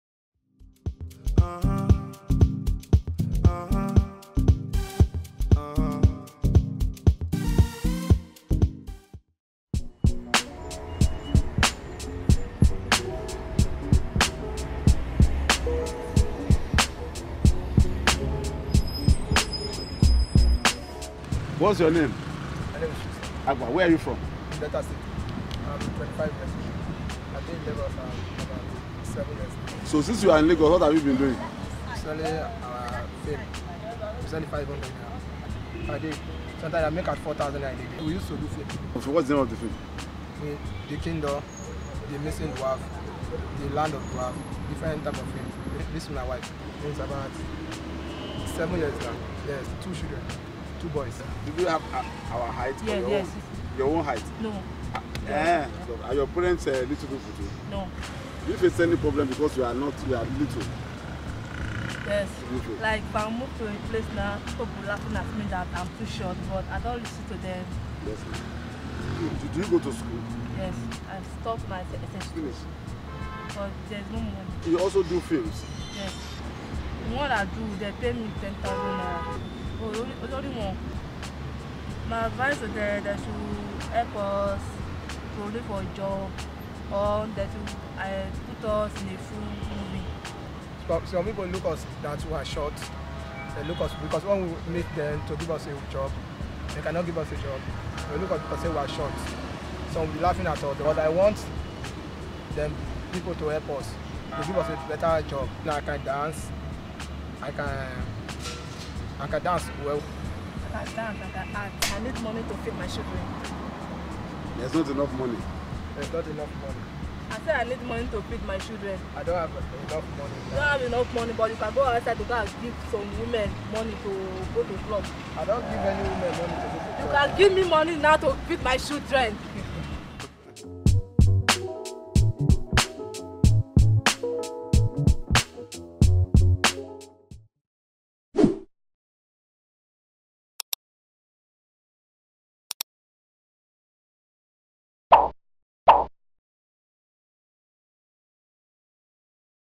Introducing [S1.EP6] : The Little People Of Lagos
Wisdom and his wife are part time actors and DVD sellers. In this 'Introducing' they tell their story and the struggles of being a little person in Lagos.